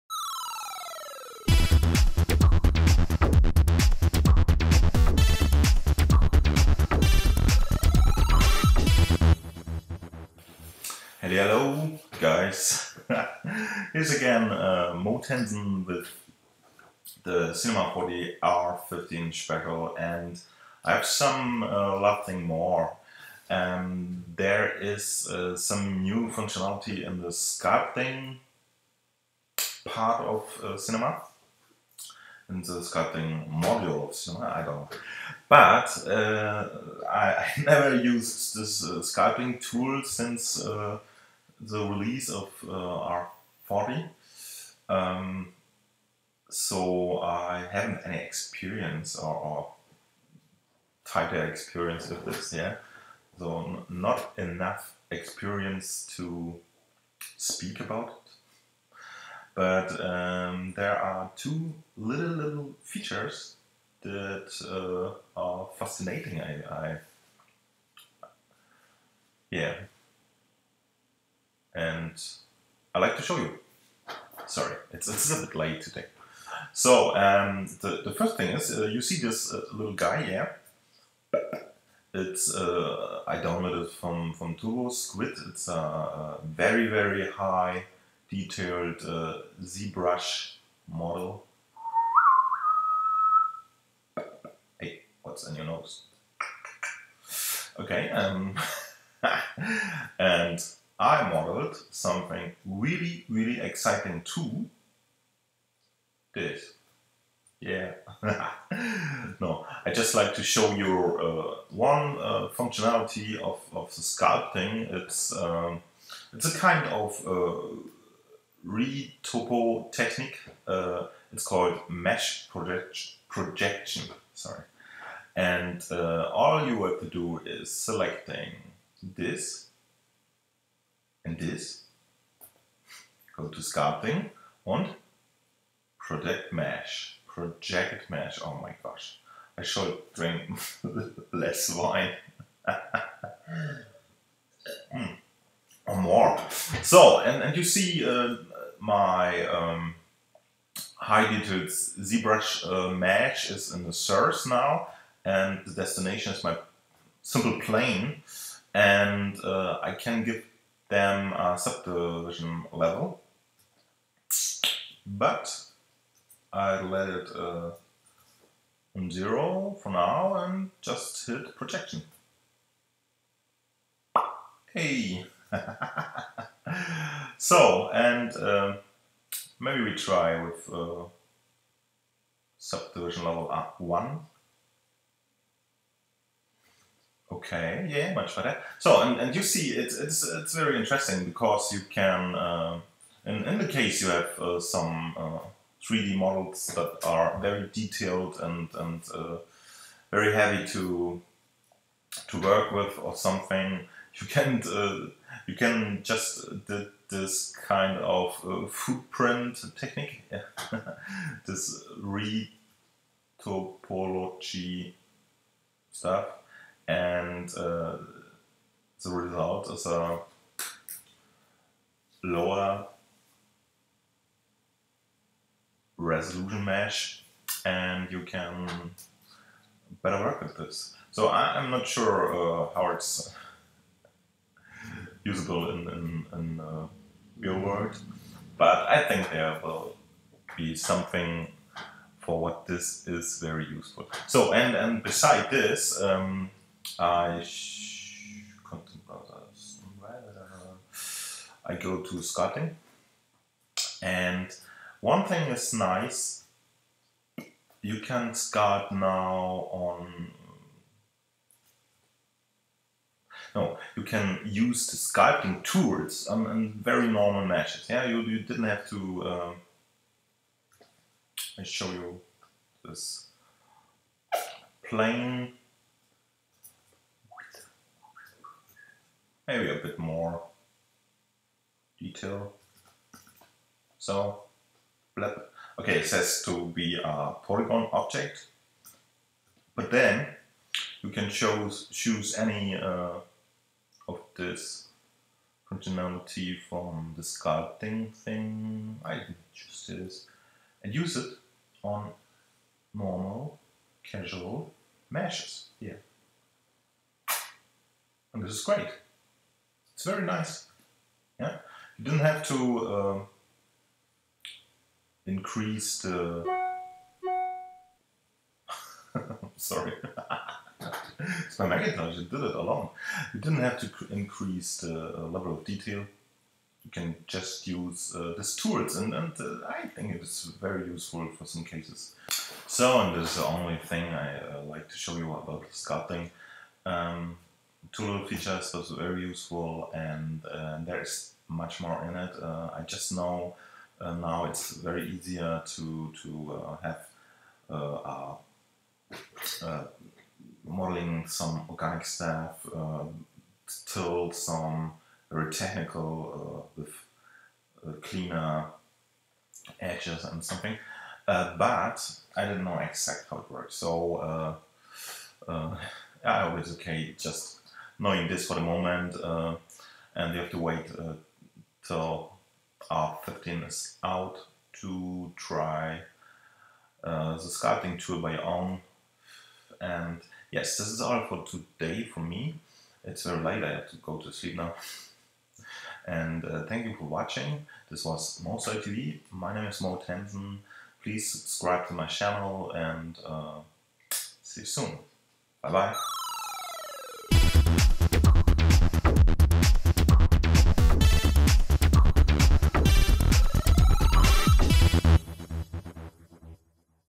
Cinema 4D r15 special 10 / sculpting news
I demonstrate the  Cinema 4D r15  new feature:  just two nice little sculpting things

best
mo